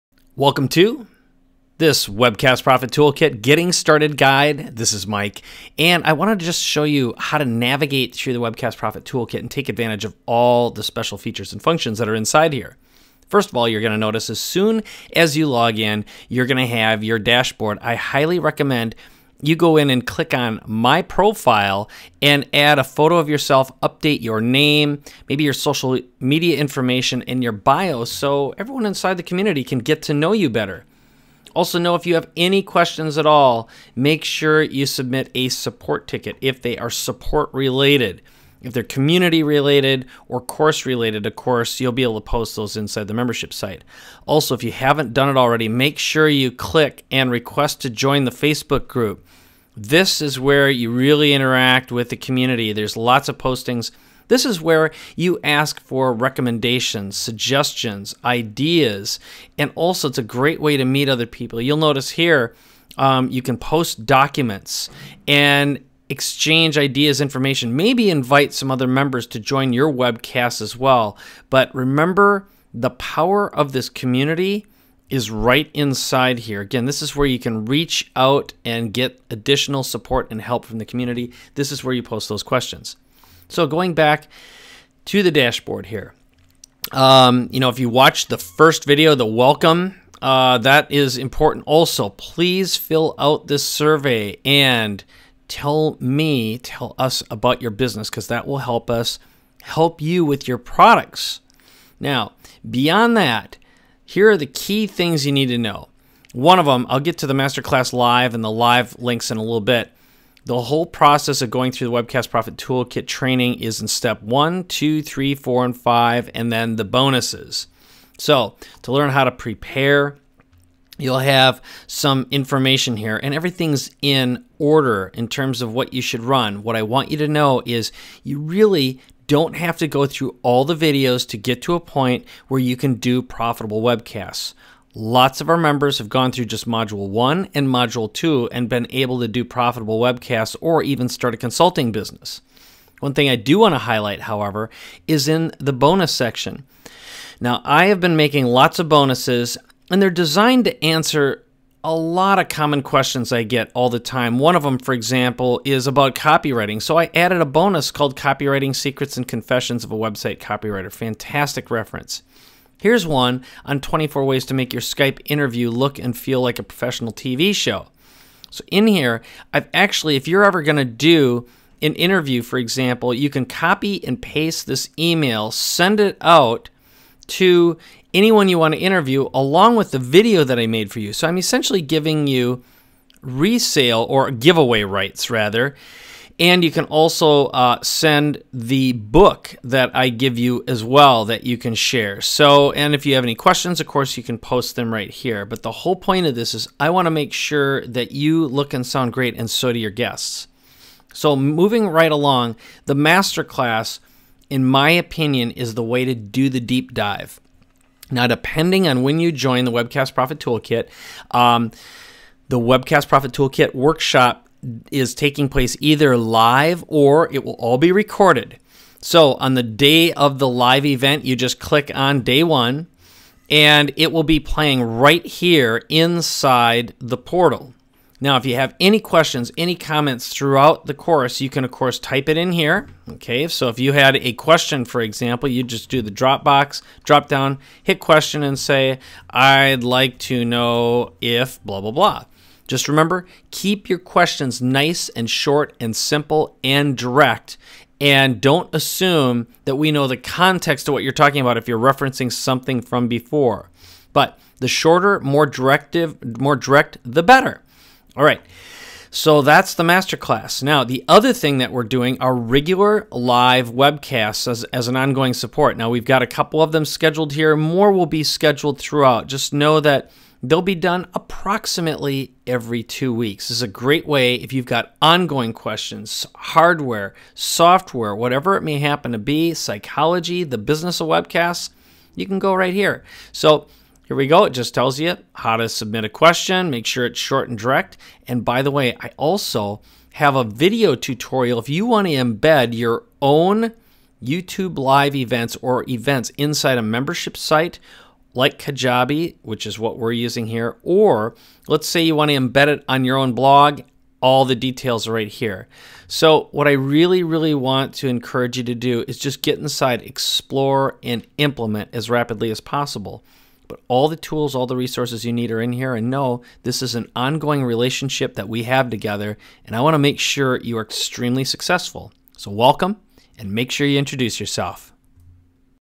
Quick Start Video Navigating the Webcast Profit Toolkit
Engage, entertain, inspire, build an audience and profit.

This is a step-by-step walkthrough of the Webcast Profit Toolkit that can be found

The Webcast Profit Toolkit is a step-by-step training program that will teach you how to create Live Interactive Online programs, shows, sales events and show you how to start or launch a business or product.

If you want to share your message, get seen, heard, viewed on any device, anytime, anywhere, that's what the system is all about.

It's for entrepreneurs, small business owners, authors, experts, speakers, consultants, coaches in any business, anywhere.

The Webcast Profit Toolkit was created by @MikeKoenigs - learn more about Mike at his personal web